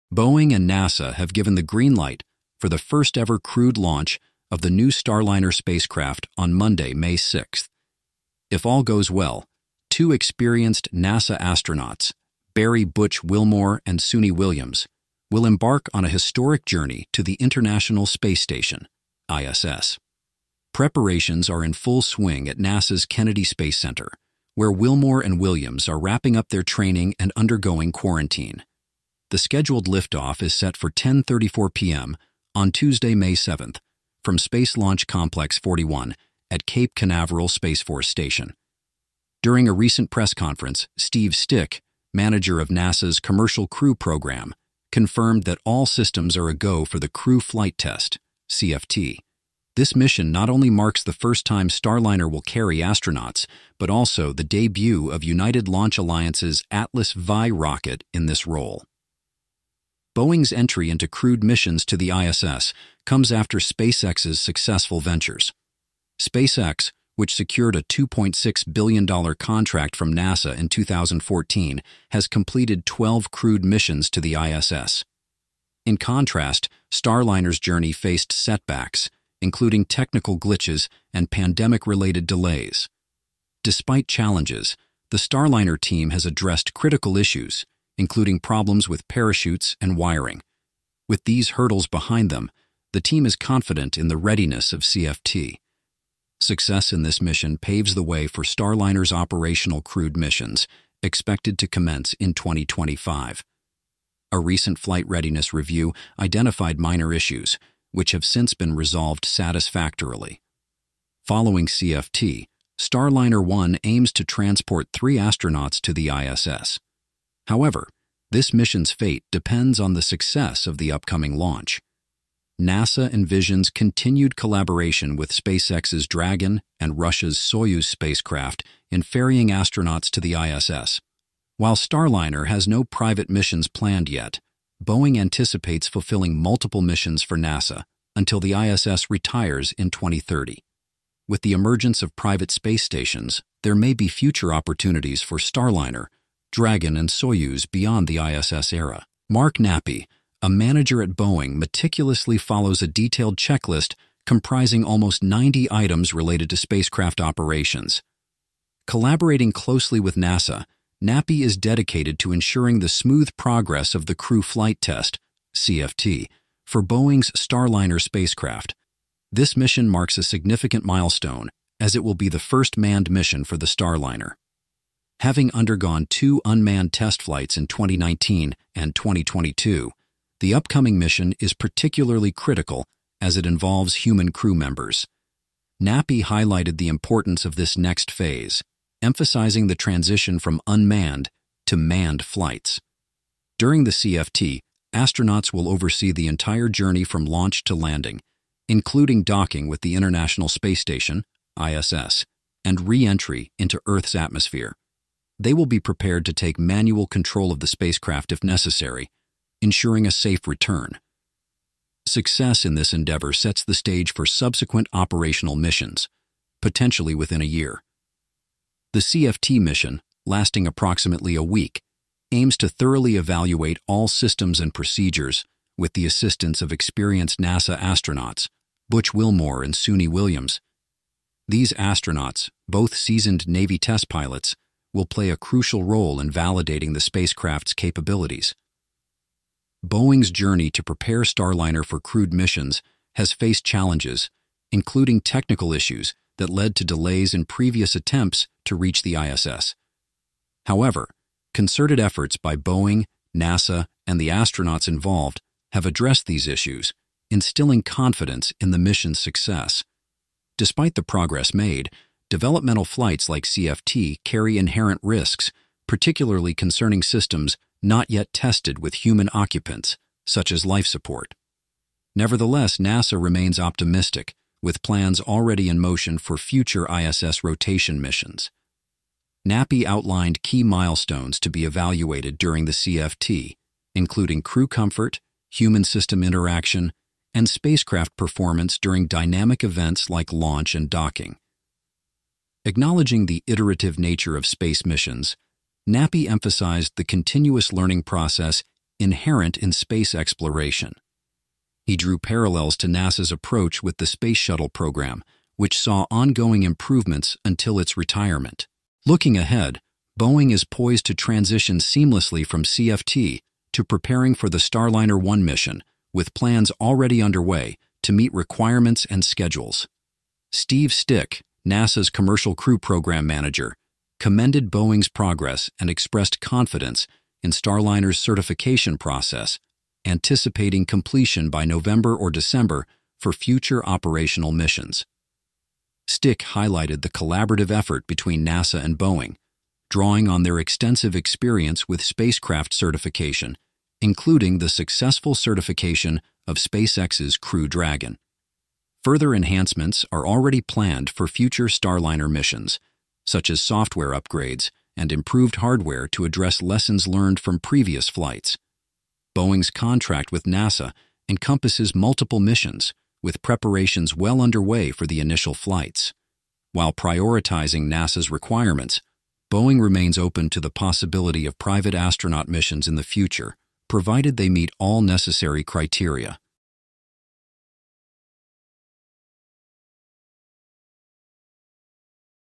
Boeing's Starliner: Embarking on a New Era of Crewed Space Exploration
"Join us as we delve into the exciting world of space exploration with Boeing's Starliner spacecraft! Discover the latest updates on its historic first-ever crewed launch to the International Space Station (ISS), marking a significant milestone in human spaceflight. Learn about the challenges, triumphs, and future possibilities as we venture beyond Earth's atmosphere.